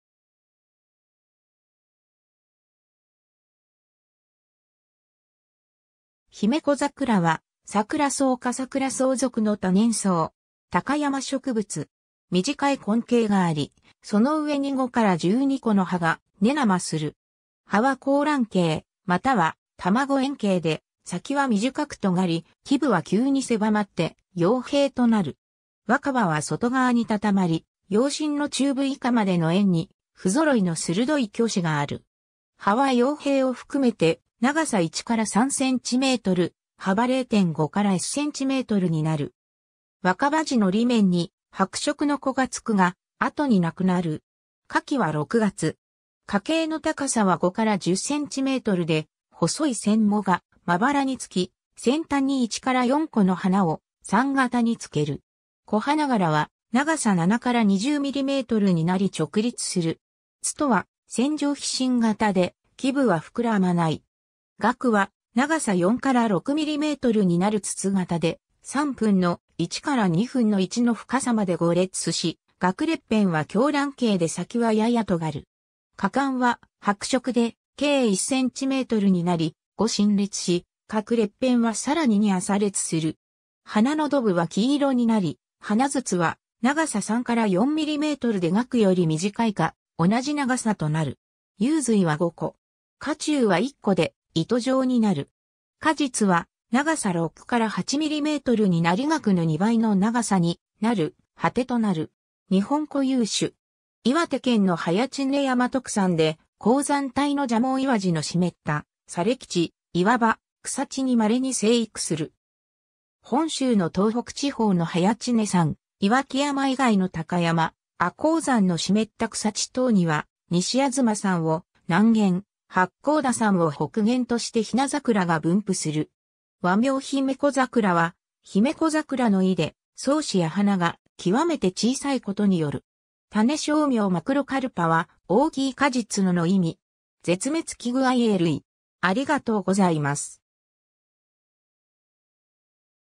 ヒメコザクラ
ヒメコザクラ（姫小桜、学名：Primula macrocarpa）は、サクラソウ科サクラソウ属の多年草。
高山植物。
 短い根茎があり、その上に5-12個の葉が根生する。
葉は広卵形または卵円形で、先は短くとがり、基部は急に狭まって葉柄となる。
若葉は外側にたたまり、葉身の中部以下までの縁に不ぞろいの鋭い鋸歯がある。
葉は葉柄を含めて長さ1-3cm、幅0.5-1cmになる。
若葉時の裏面に白色の粉がつくが、後になくなる。
 花期は6月。
花茎の高さは5-10cmで細い腺毛がまばらにつき、先端に1-4個の花を散形につける。
小花柄は長さ7-20mmになり直立する。
苞は線状披針形で、基部はふくらまない。
萼は長さ4-6mmになる筒型で、3分の1から2分の1の深さまで5裂し、萼裂片は狭卵形で先はややとがる。
花冠は白色で径1cmになり、5深裂し、各裂片はさらに2浅裂する。
花喉部は黄色になり、花筒は長さ3-4mmで萼より短いか同じ長さとなる。
雄蕊は5個。
花柱は1個で糸状になる。
果実は長さ6-8mmになり萼の2倍の長さになる蒴果となる。
 日本固有種 。
岩手県の早池峰山特産で、高山帯の蛇紋岩地の湿った砂礫地、岩場、草地にまれに生育する。
 本州の東北地方の早池峰山、岩木山以外の高山、亜高山の湿った草地等には、西吾妻山を南限、八甲田山を北限としてヒナザクラが分布する。
 和名ヒメコザクラは、姫小桜の意で、草姿や花がきわめて小さいことによる。
種小名 macrocarpa は、「大きい果実の」の意味。
 絶滅危惧IA類 (CR)（環境省レッドリスト） （2017年環境省レッドリスト）